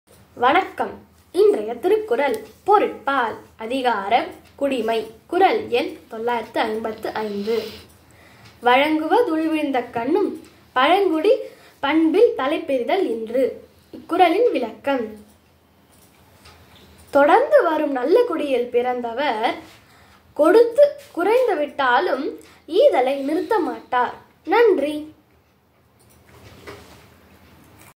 குறள் எண் - 955 திருக்குறள் பொருட்பால் அதிகாரம் - 96. குடிமை
வழங்குவ துள்வீழ்ந்தக் கண்ணும் பழங்குடி
பண்பில் தலைப்பிரிதல் இன்று